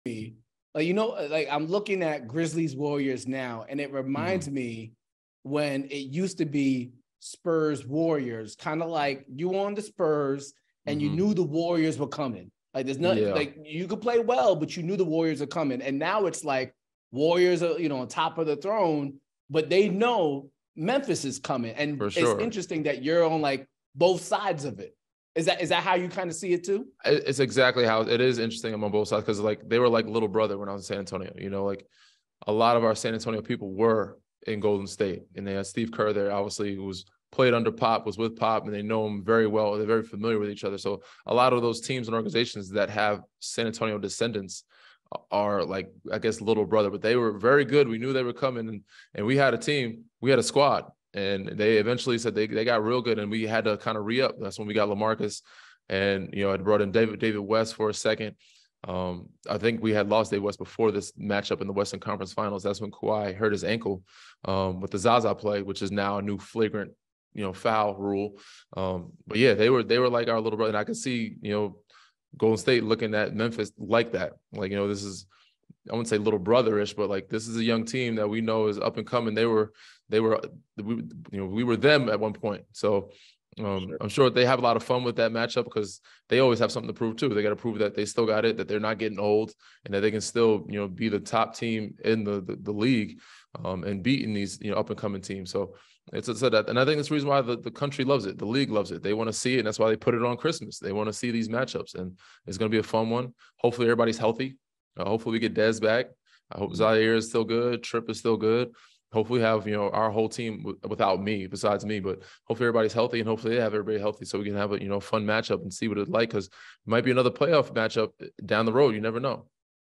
Do The Warrior's Know What Memphis Is Preparing?
Ziaire Williams joins Danny & Harrison in this episode with some tough questions to answer like why he chose Stanford over UNC. He also shares with the guys about his journey through rehab, having military parents who shaped him, and his excitement for his first Christmas game. Tune in for more.